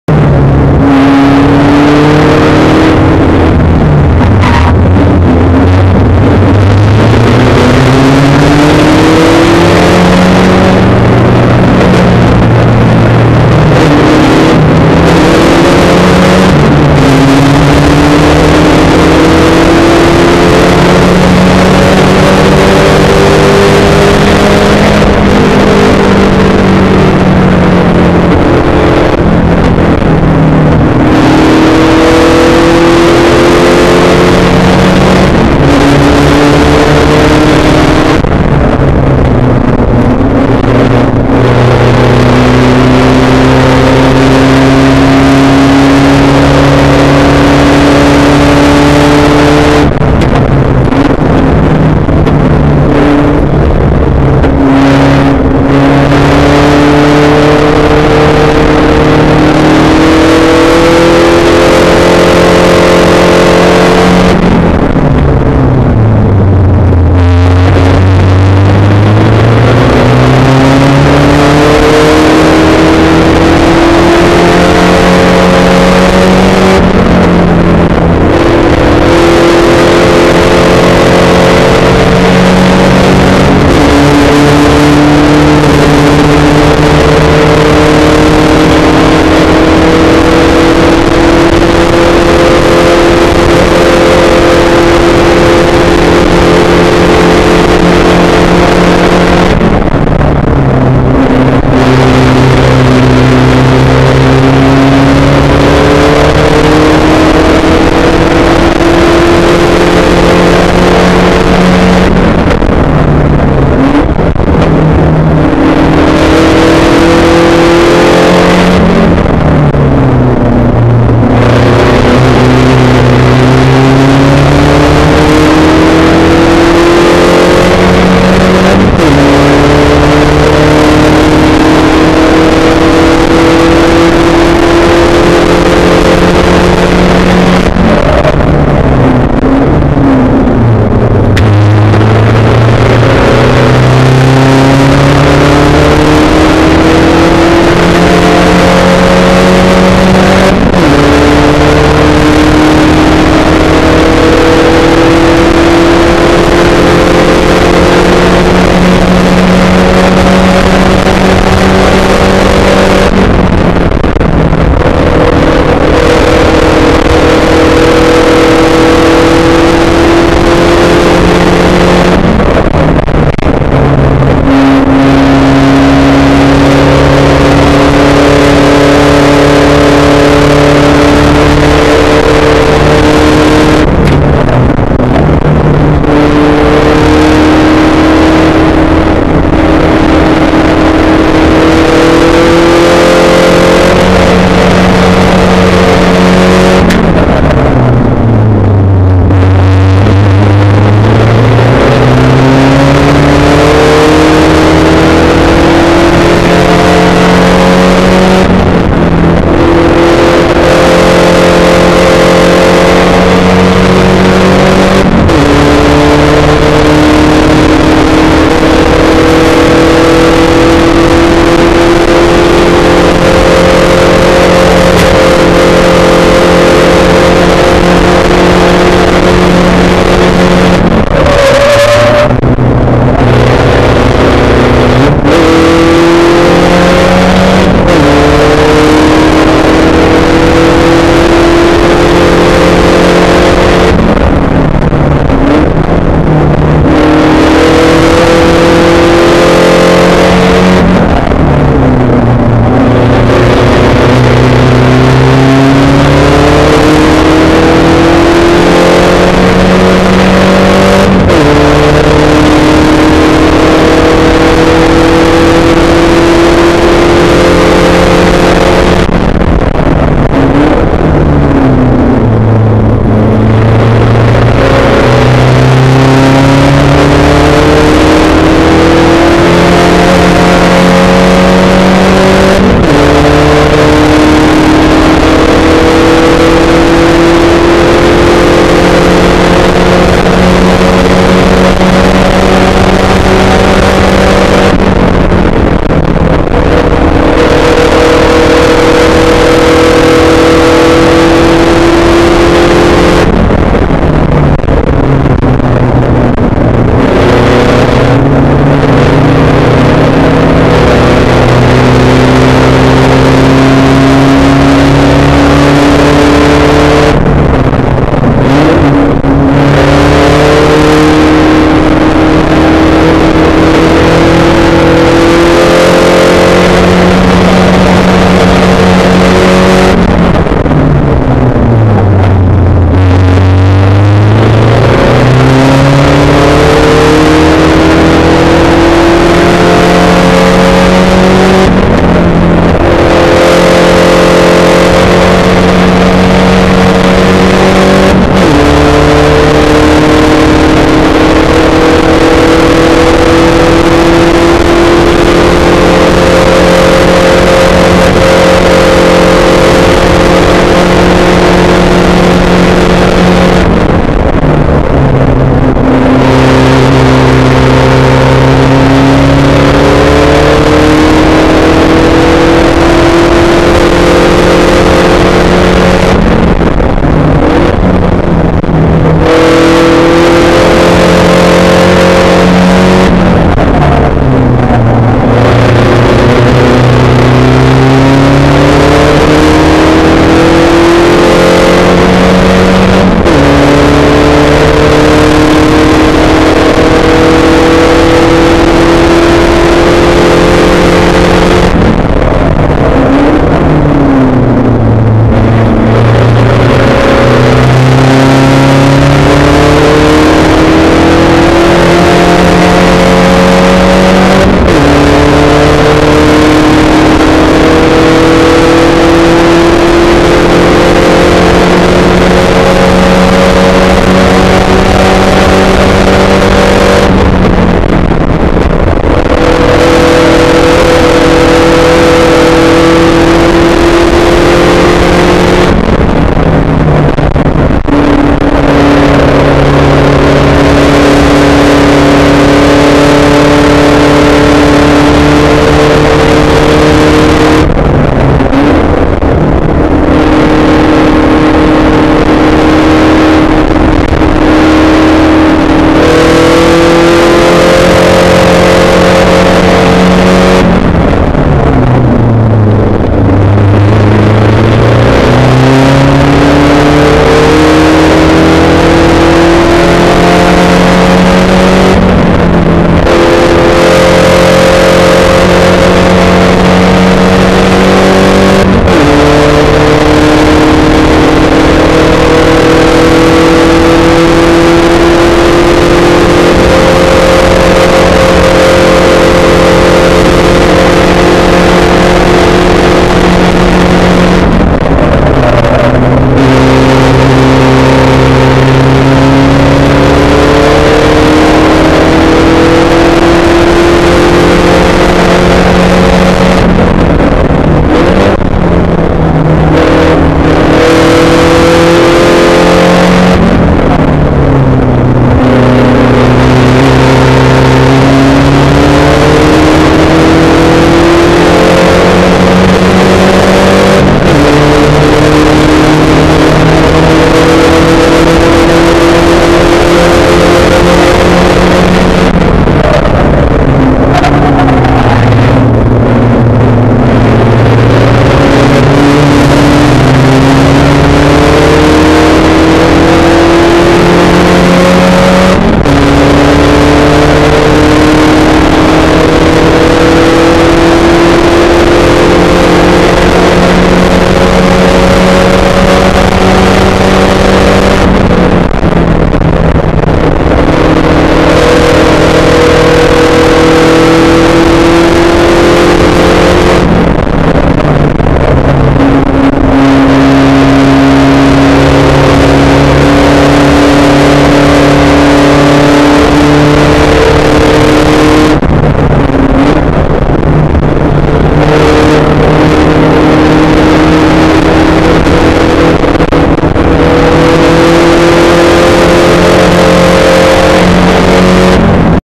Opening laps SFR Regional #4 Thunderhill Spec racer Ford
The opening laps of the second SRF race of the double regional weekend at Thunderhill, May 25th, 2008